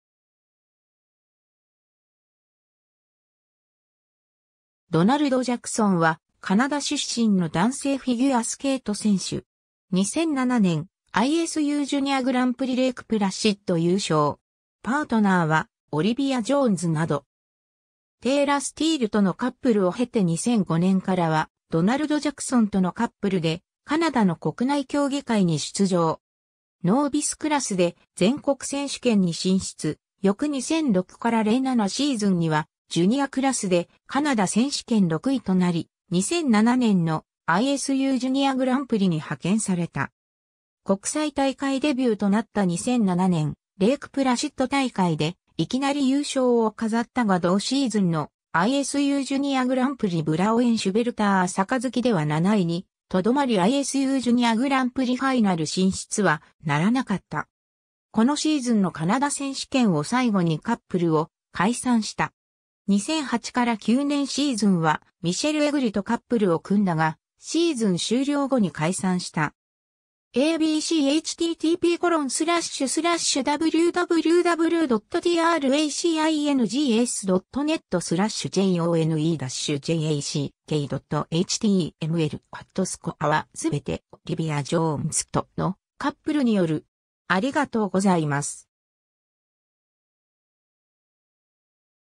ドナルド・ジャクソン (1986年生)
ドナルド・ジャクソン（英語: Donald Jackson, 1986年9月26日 - ）は、カナダ出身の男性フィギュアスケート選手（ペア）。
2007年ISUジュニアグランプリ レークプラシッド優勝。
パートナーはオリビア・ジョーンズなど。
 テイラー・スティールとのカップルを経て2005年からはドナルド・ジャクソンとのカップルでカナダの国内競技会に出場。
ノービスクラスで全国選手権に進出、翌2006-07シーズンにはジュニアクラスでカナダ選手権6位となり、2007年のISUジュニアグランプリに派遣された。
 国際大会デビューとなった2007年レークプラシッド大会でいきなり優勝を飾ったが同シーズンのISUジュニアグランプリ ブラオエン・シュベルター杯では7位にとどまりISUジュニアグランプリファイナル進出はならなかった。
このシーズンのカナダ選手権を最後にカップルを解散した。
 2008-09年シーズンはMichelle Egliとカップルを組んだが、シーズン終了後に解散した。
^ a b c ^ スコアは全てオリビア・ジョーンズとのカップルによる。